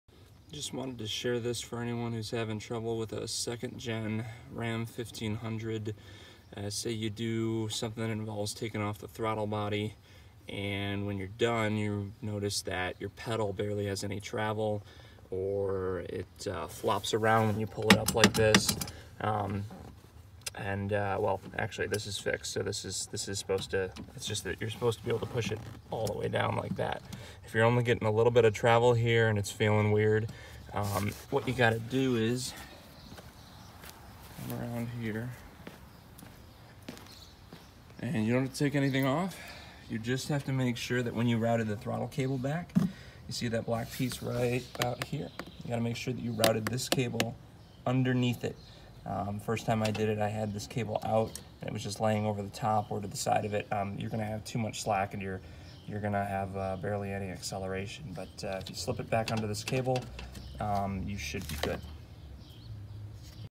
Dodge Ram Throttle Cable too loose Fix
For anyone who puts the throttle body back together and notices issues with the gas pedal/throttle not having any travel or acting weird. Couldn’t find anyone else’s so I figured I’d upload mine.